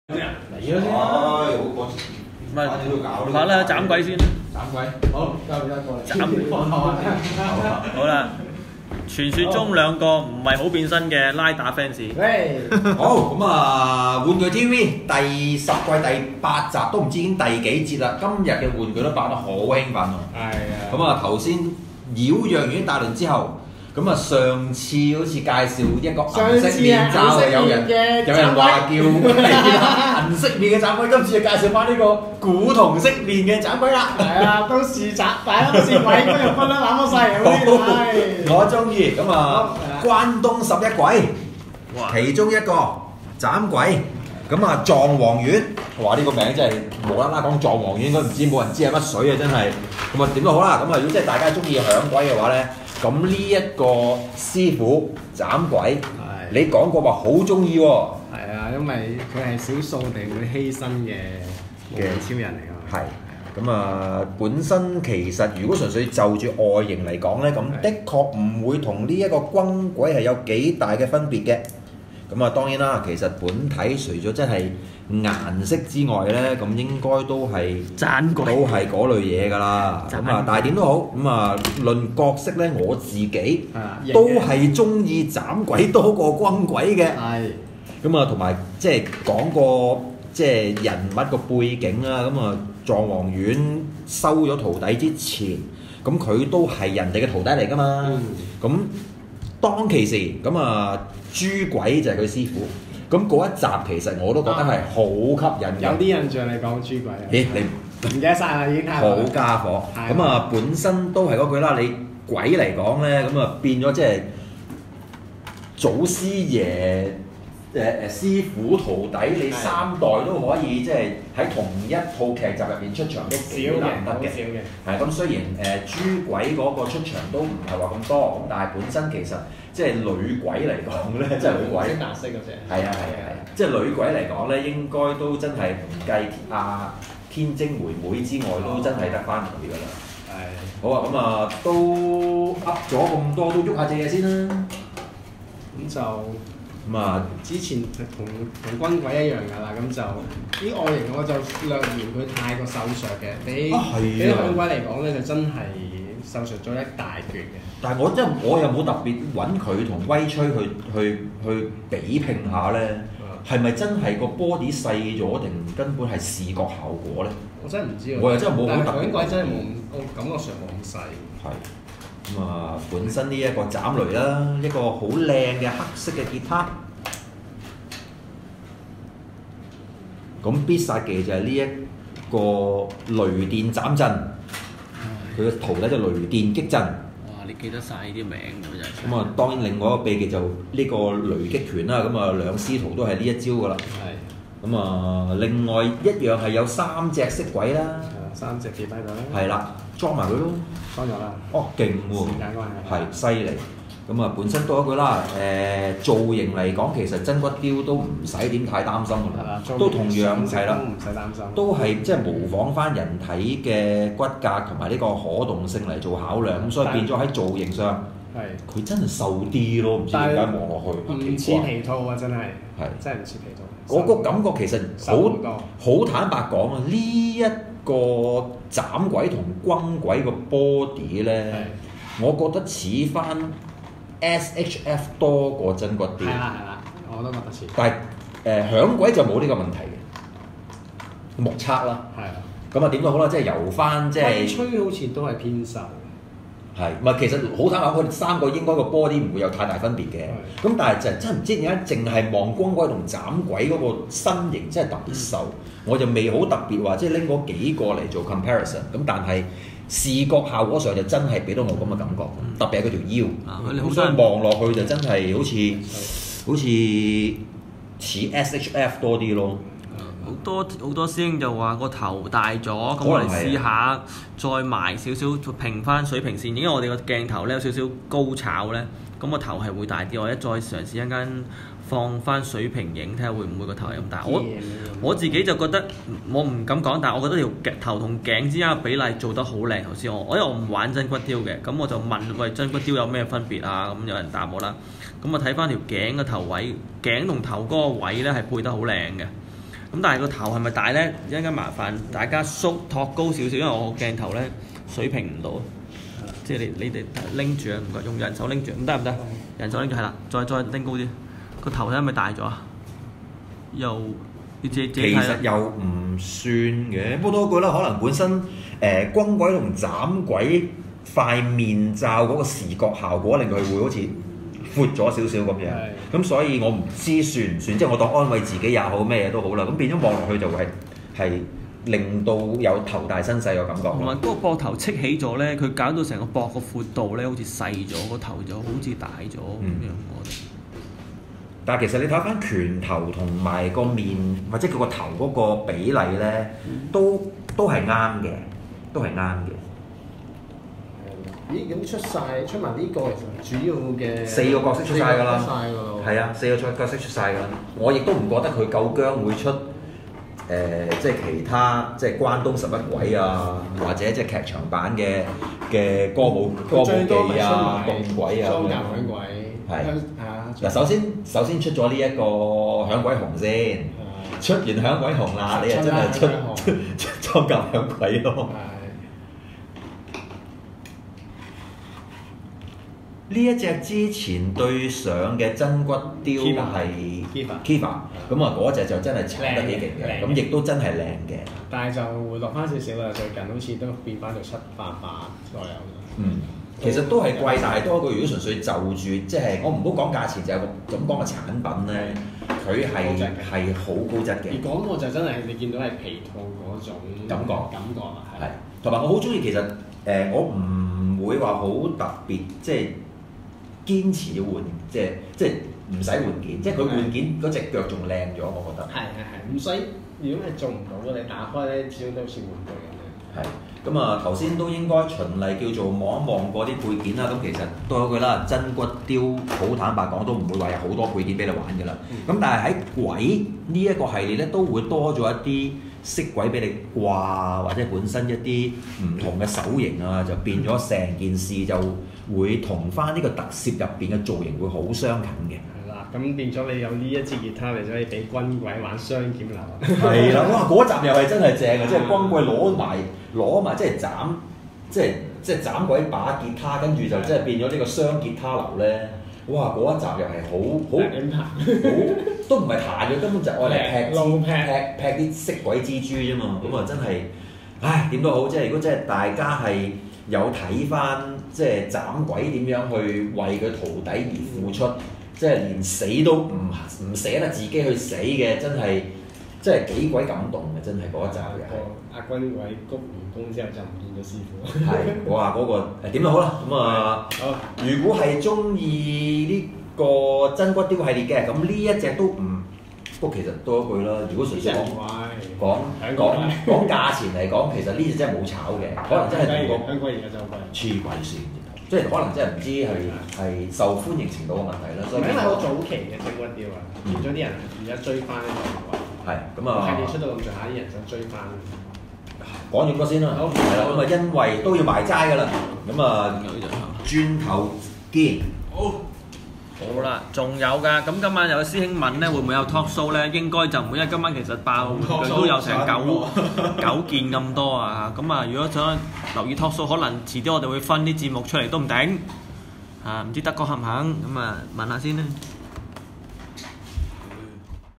TOYSTV S10 E8「爆玩具」Bandai S.H.Figuarts SHF 真骨彫 斬鬼Masked Rider ZANKI SHINKOCCHOUSEIHOU Unbox
Production: TOYSTV Production Team
Program host/Guest: DMS, Curtis, Elman, Dick, David, Bryan
Produced by: Bryan, Dick